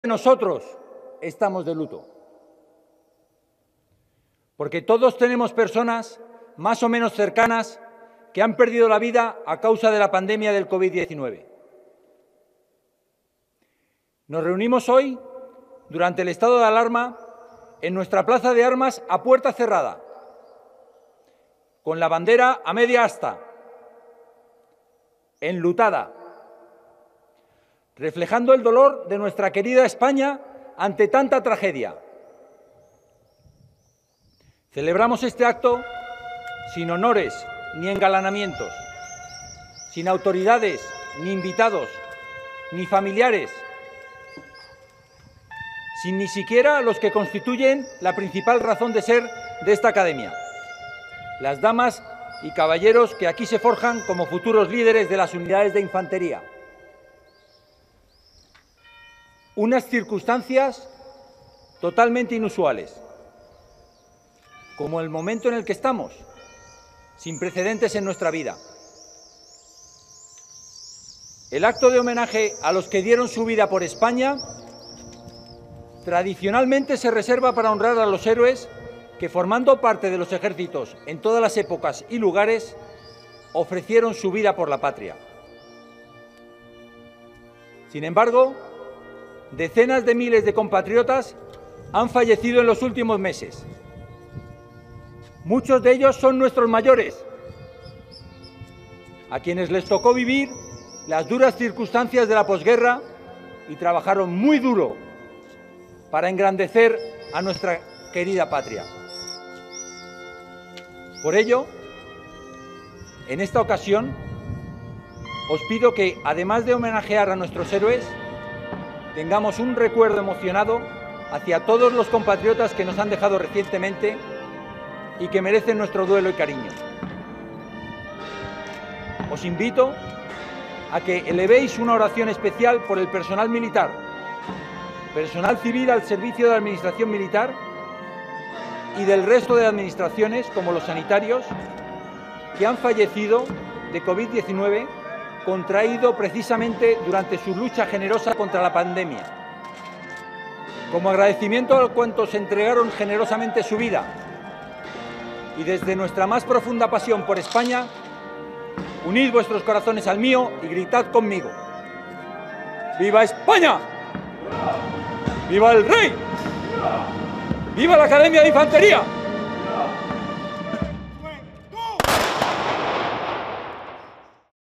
Homenaje de la Academia de Infantería a los muertos por COVID-19
Homenaje celebrado el viernes 29 de mayo de 2020 en la Academia de Infantería del Ejército de Tierra de España, con sede en la ciudad de Toledo, a los fallecidos por la pandemia de coronavirus. El vídeo incluye las palabras pronunciadas por el director de la Academia, el coronel Antonio Armada, en homenaje a los que han muerto a causa de esta epidemia.